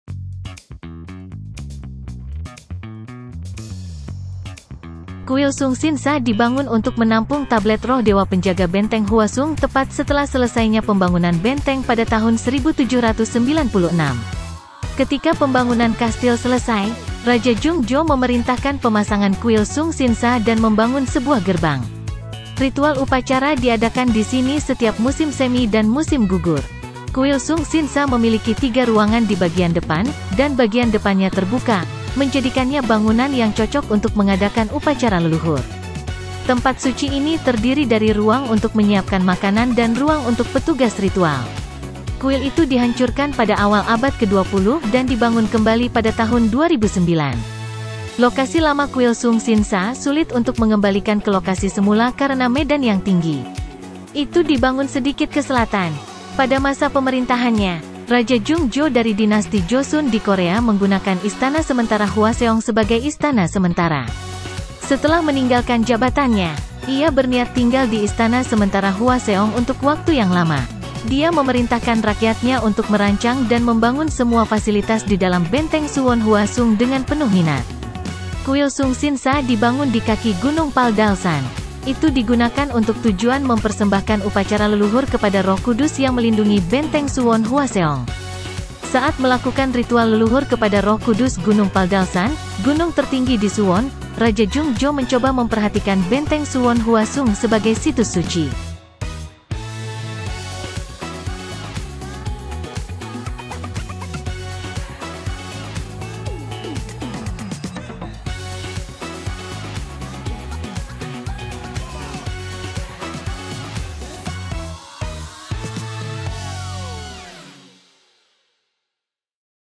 Versi Indonesia - Sung Sin Sa, Su Won Hwa Sung - Tempat Populer Korea untuk Tur Anda -
Versi Indonesia - Sung Sin Sa, Su Won Hwa Sung - Tempat Populer Korea untuk Tur Anda - CEO PM

Indonesian Version - Sung Sin Sa, Su Won Hwa Sung - Korean Hot Place for Your Tour - CEO PM